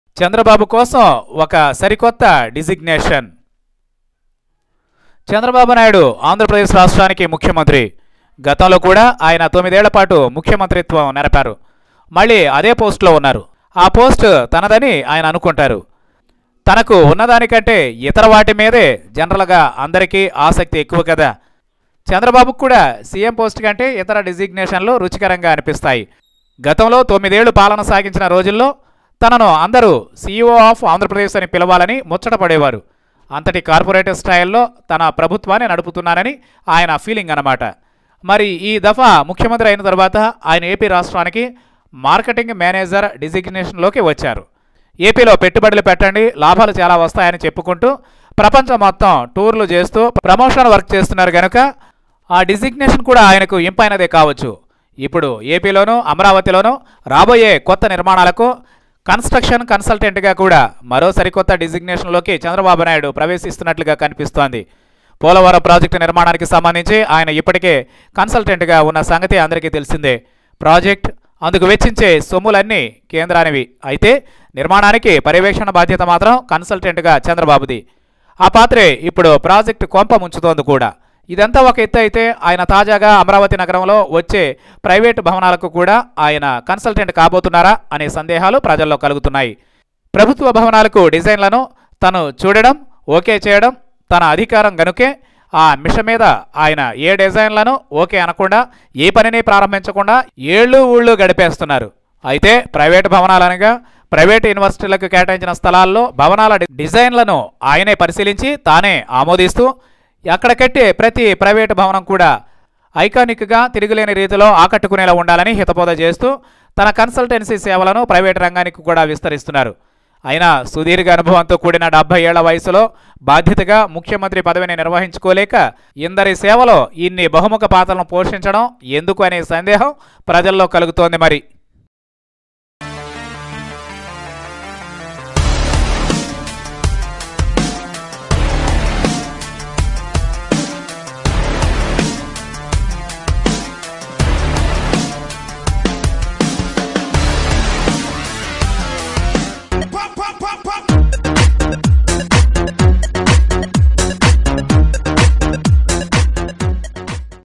చంద్రబాబు కొత్త డిజిగ్నేషన్ వింటే షాకవ్వాల్సిందే..!| CM Chandrababu New designation Will Shock You.!
CM Chandrababu New designation Will Shock You.!
CM Chandrababu Naidu Decided to give Consultancy Services to Private Companies..! Watch this video to know Chandrababu's new designation.